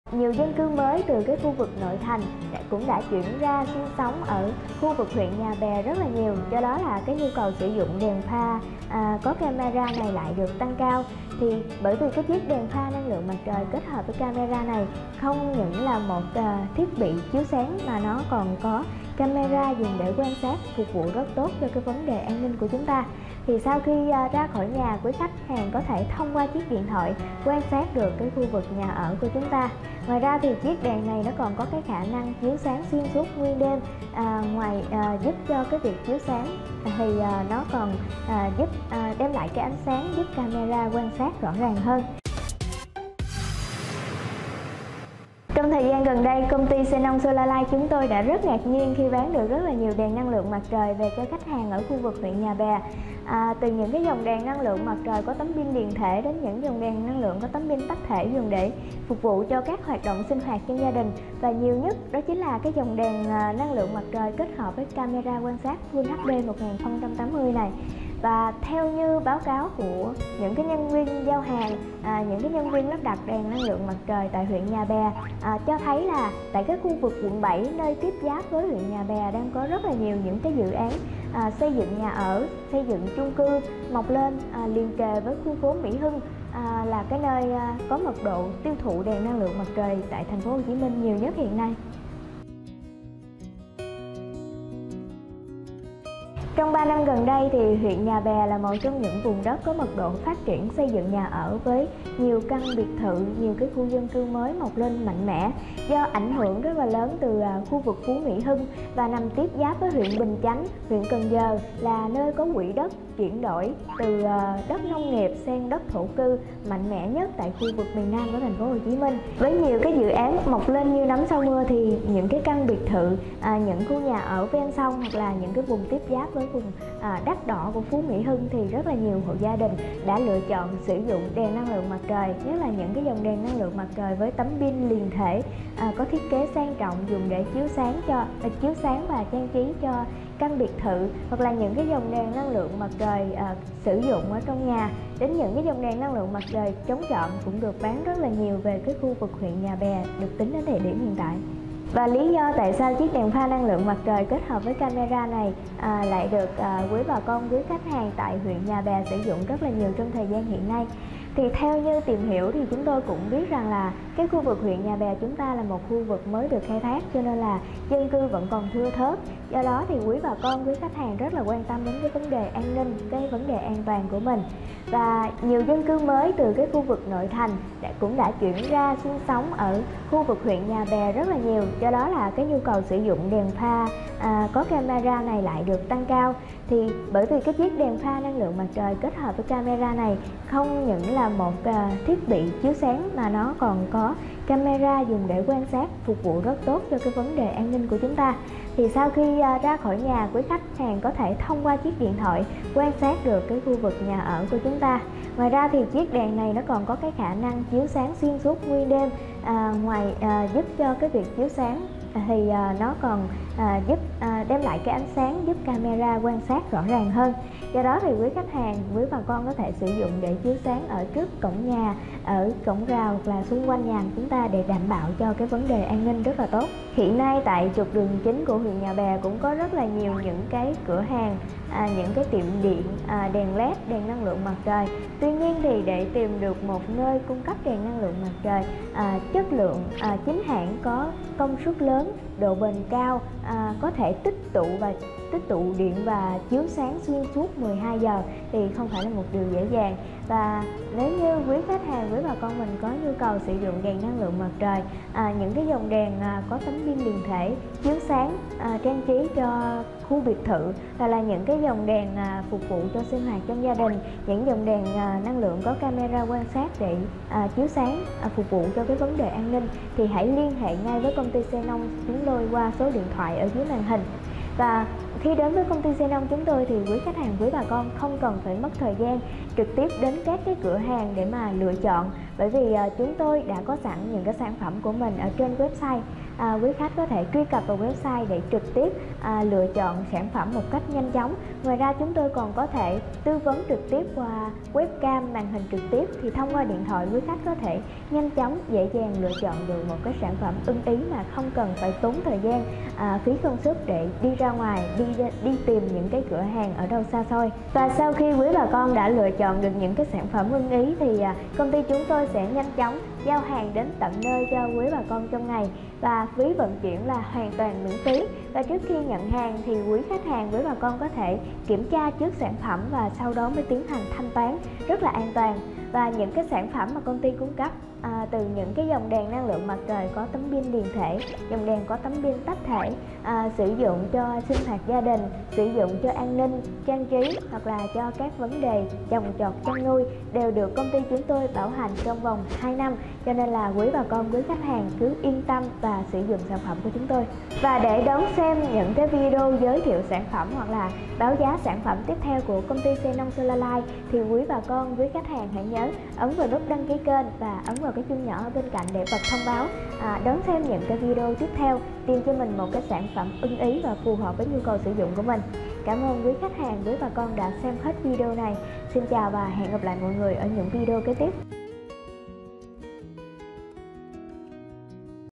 Đèn năng lượng mặt trời Huyện Nhà Bè| đèn năng lượng có camera quan sát Full HD1080
Trong thời gian gần đây Công ty Xenon Solar Light chúng tôi rất ngạc nhiên khi bán được rất nhiều đèn năng lượng mặt trời về cho khách hàng ở Huyện Nhà Bè trong đó có nhiều loại đèn năng lượng mặt trời dạng liền thể và loại pin tách thể sử dụng trong sinh hoạt gia đình, đặc biệt nhiều nhất là loại đèn năng lượng có camera quan sát Full HD1080. Báo cáo từ các nhân viên giao hàng và lắp đặt đèn năng lượng tại Huyện Nhà Bè cho thấy rằng xung quanh khu vực Quận 7 tiếp giáp với Huyện Nhà Bè có hàng loạt các dự án xây dựng nhà ở và chung cư mọc lên liền kề khu phố Phú Mỹ Hưng là nơi có mật độ tiêu thụ điện năng lượng mặt trời rất lớn tại thành phố Hồ Chí Minh.
HỆ THỐNG PHÂN PHỐI ĐÈN NĂNG LƯỢNG
Cửa Hàng: 4380B Nguyễn Cửu Phú, P Tân Tạo A, Q bình Tân. TPHCM